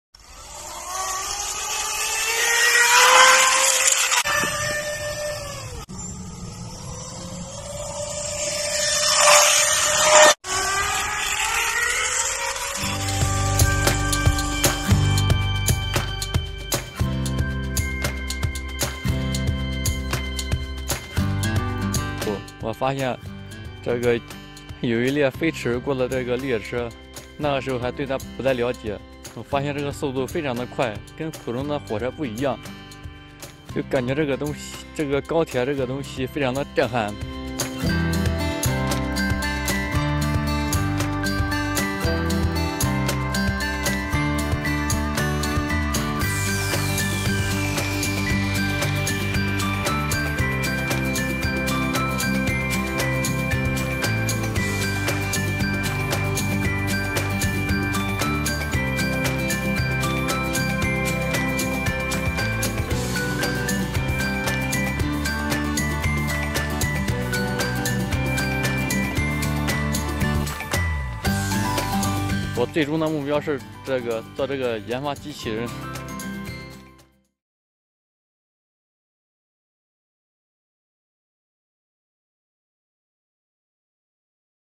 Chinese college student builds model railways in vegetable plot| CCTV English
Zhang Yadong, a student from Zhoukou city in Central China's Henan province, has built miniature infrastructure including railways, tunnels and bridges in his vegetable plot at home. His formidable hands-on skills have wowed Chinese netizens.